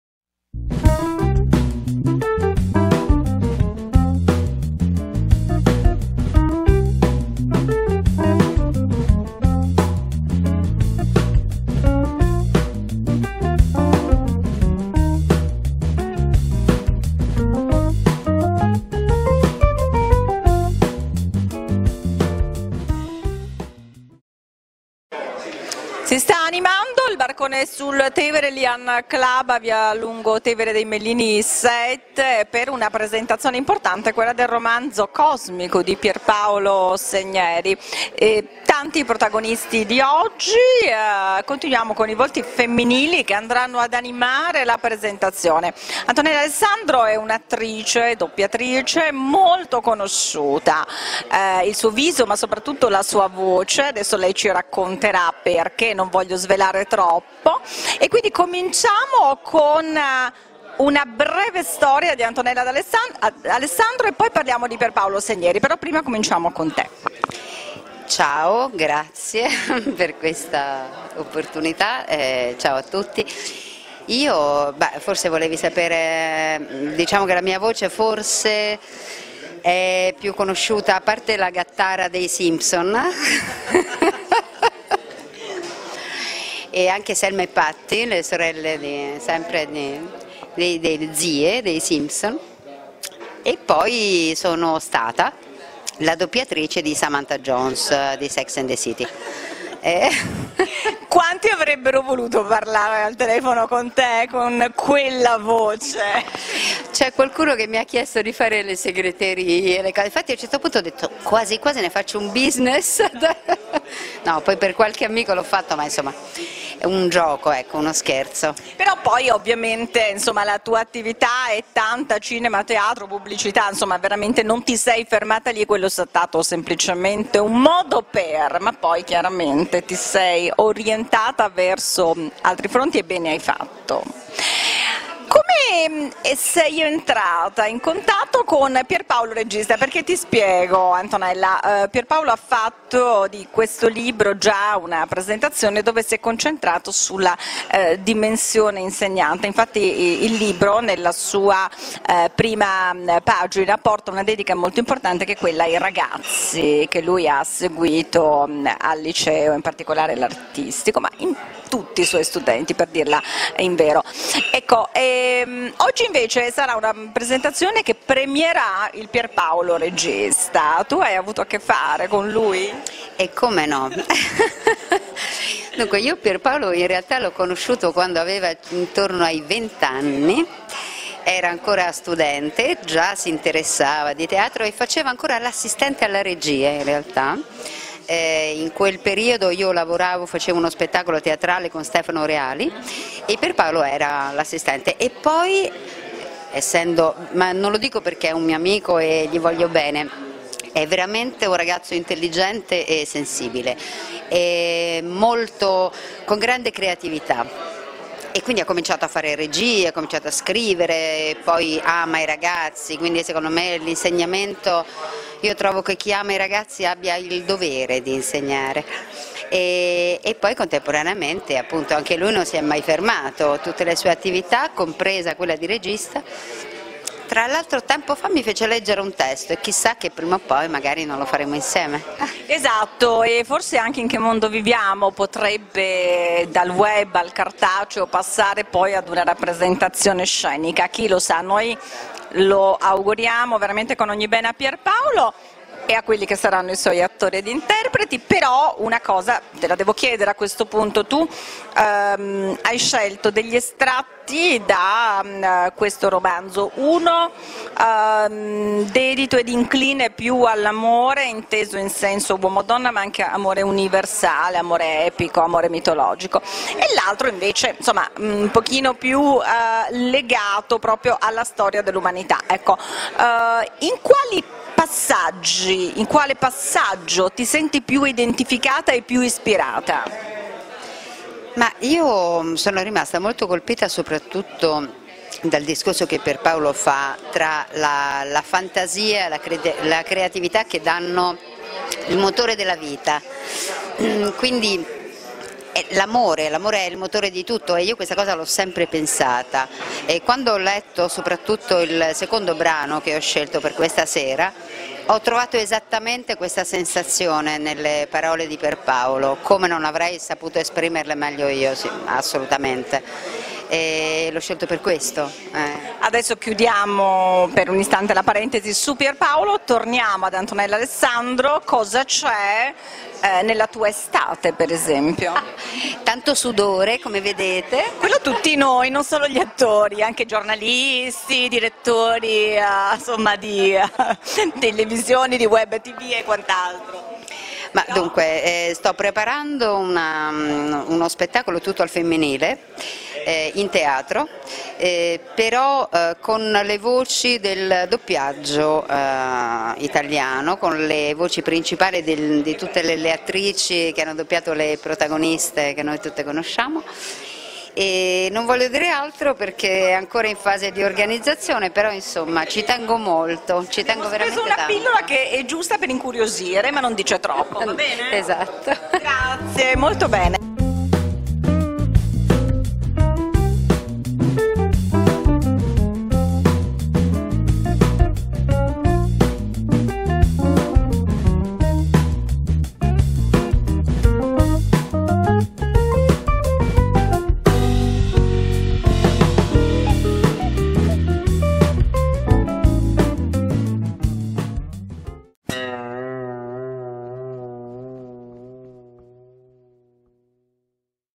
Antonella Alessandro - "IN CHE MONDO VIVIAMO", il viaggio
Presentazione del libro "In che mondo viviamo" di Pier Paolo Segneri - L'Erudita editore.

Intervista di CAMILLA NATA ad ANTONELLA ALESSANDRO, attrice e doppiatrice.